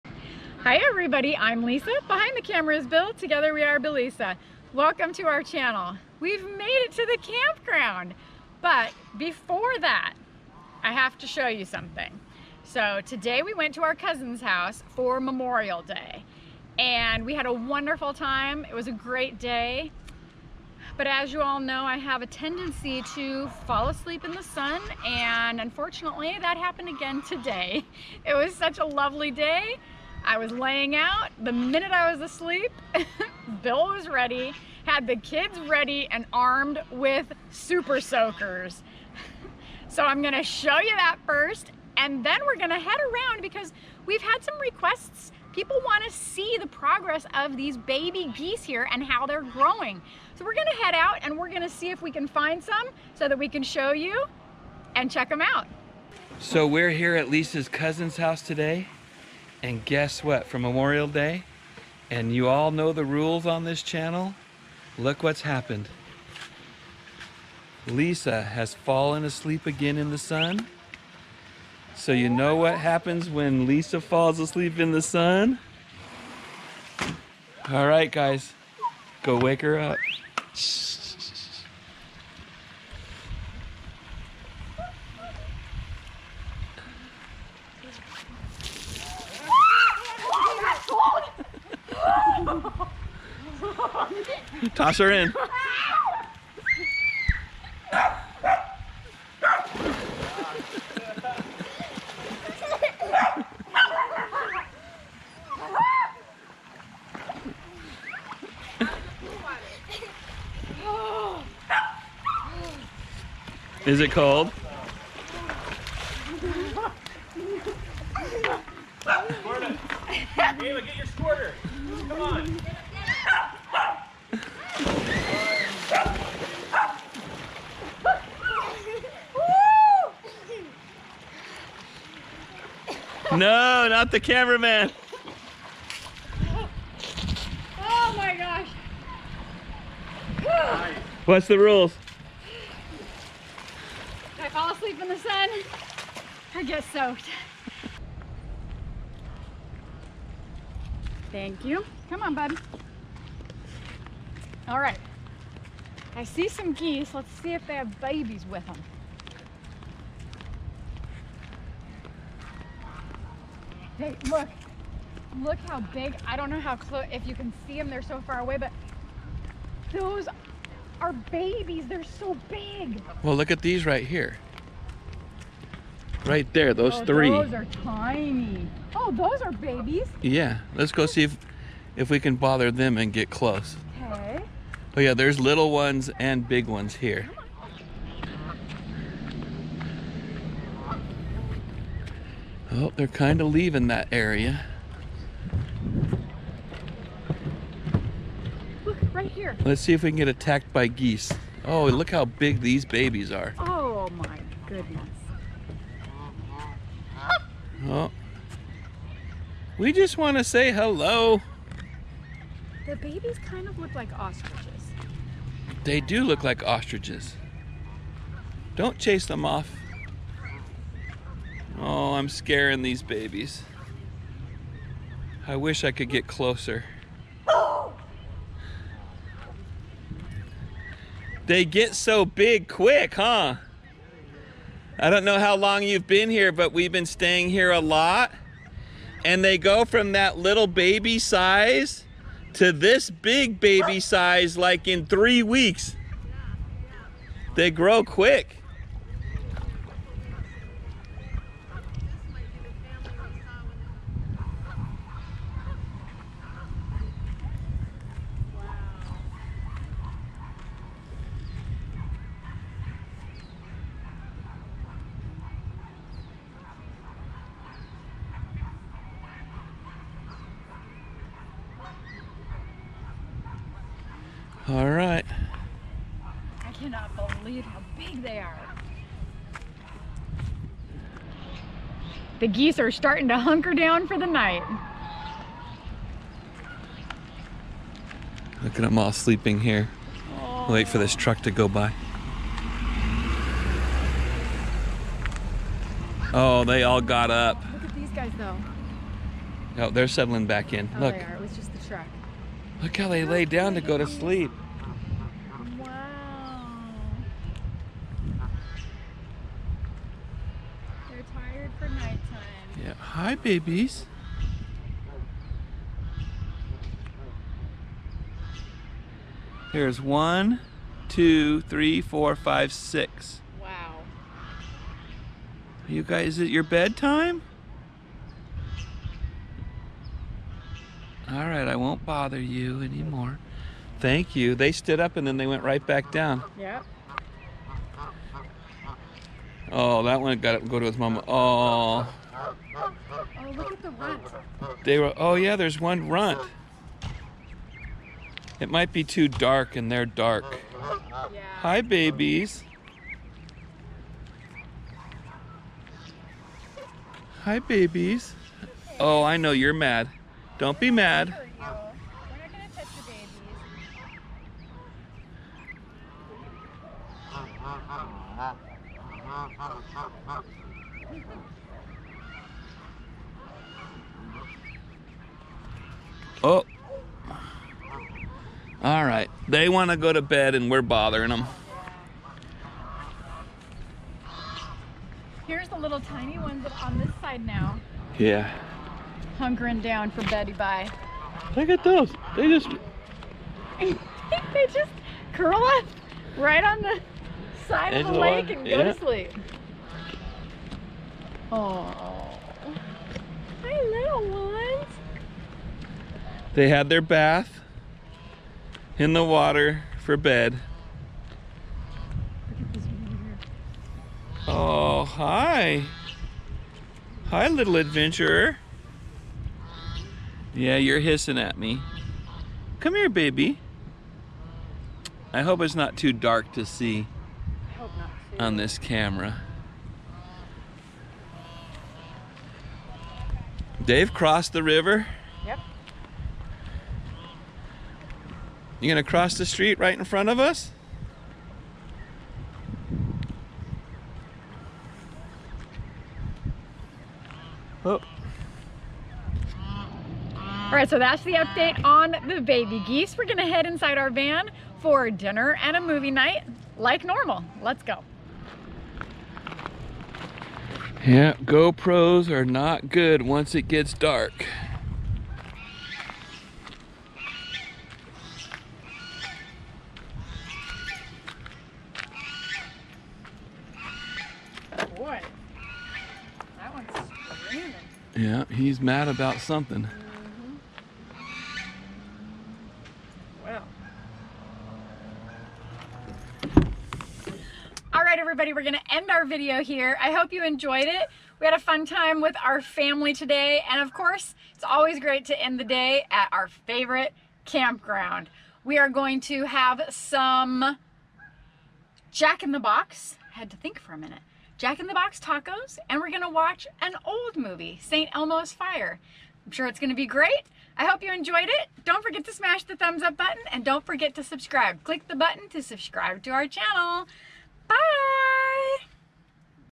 Lisa Falls Asleep In The Sun Again. You Know What That Means. Baby Geese Update
Lisa fell asleep in the sun again. This time at her cousin's home. You know what that means. Also baby geese update at Thousand Trails Wilderness Lakes. have an awesome day, Billisa